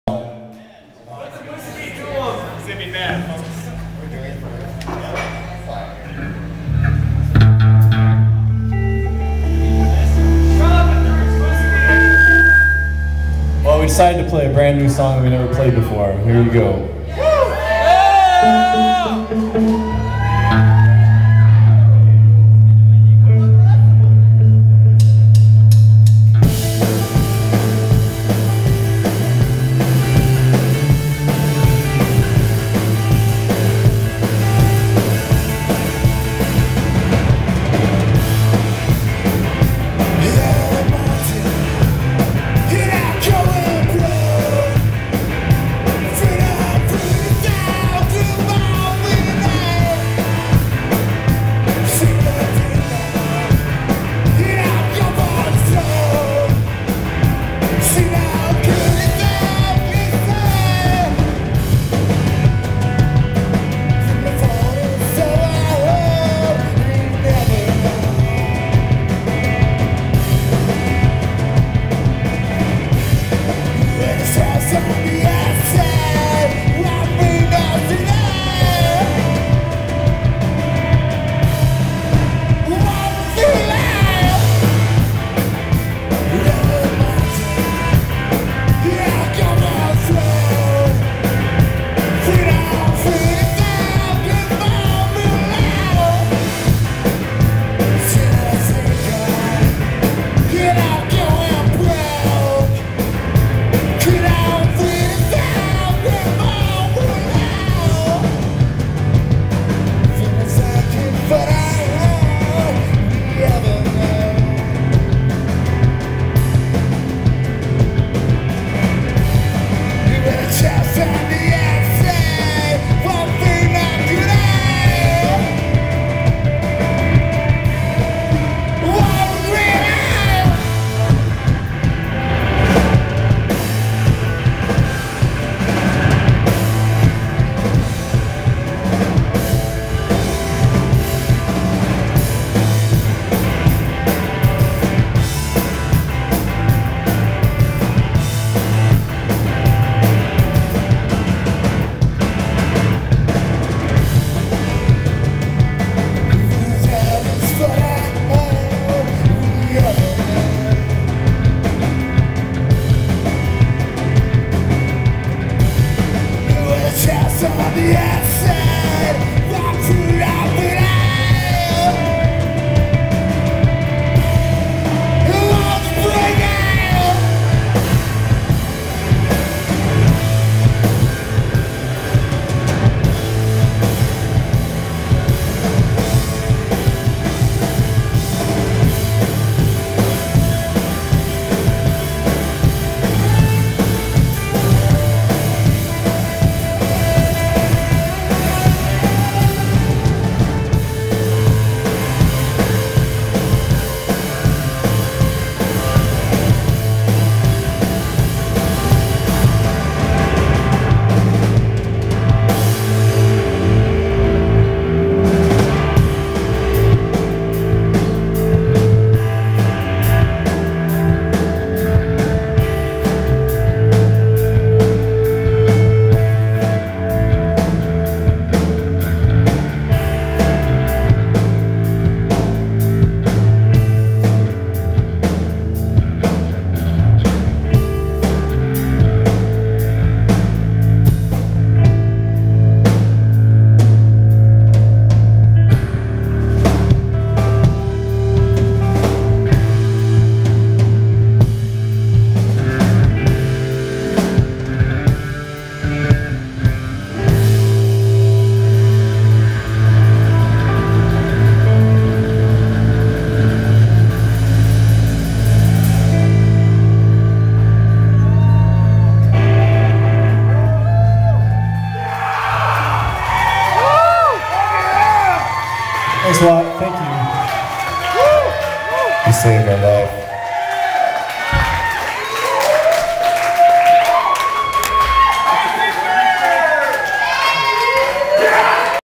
Disguised as Birds - A brand new song we've never played before(live)
Disguised as Birds play a new song that they've never played before at Stonefly in Milwaukee WI shot on a flipcam